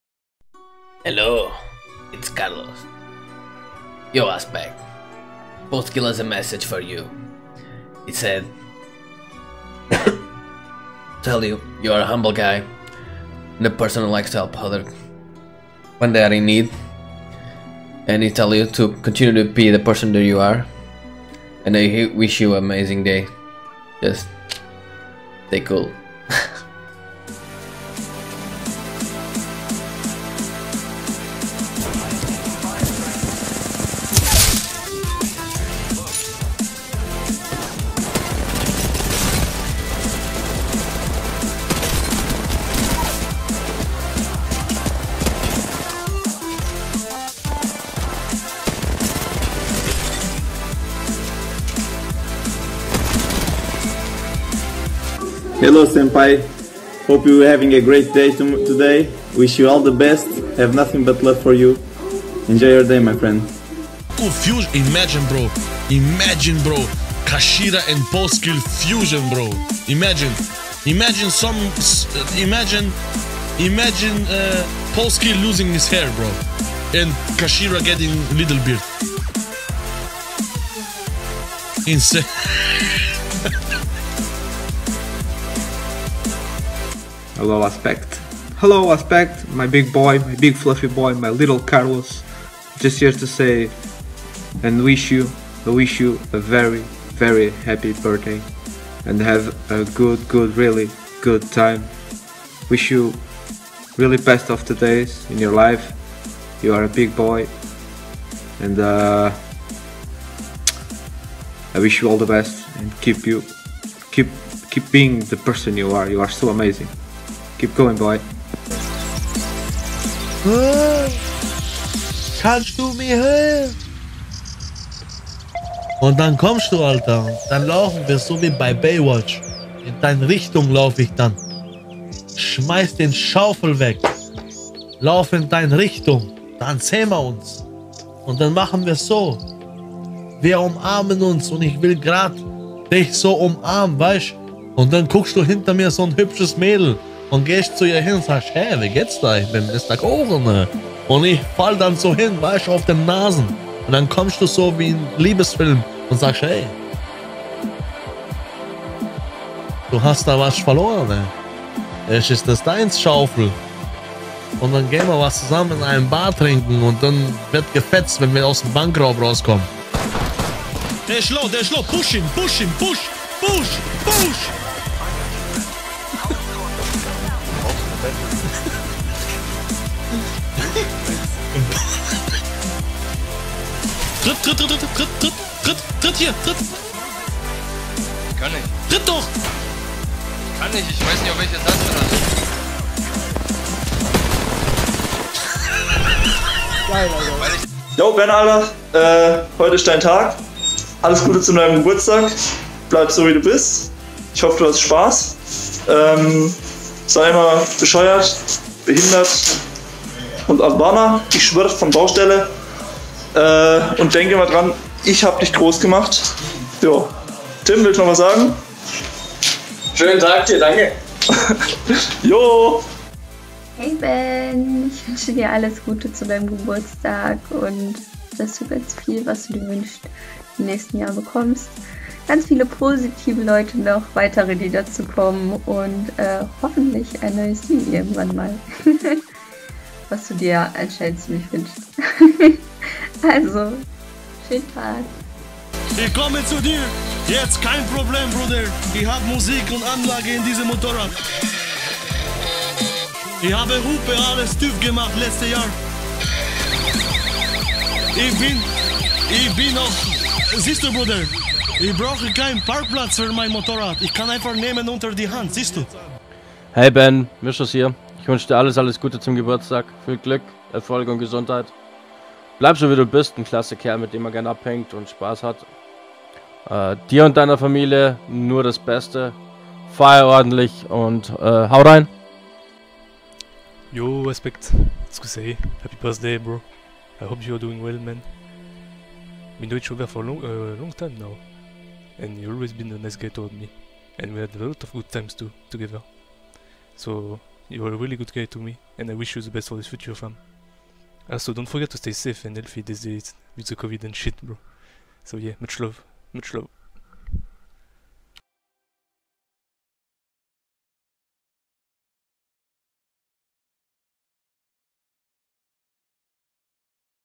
Azpectgeburtstag
Nur für dich zu deinem Geburtstag Bro Ich Wünsche dir Viel Kraft, Gesundheit und Erfolg weiterhin.

Wir zusammen als Community werden immer Hinter dir stehen.
Bleib so wie du bist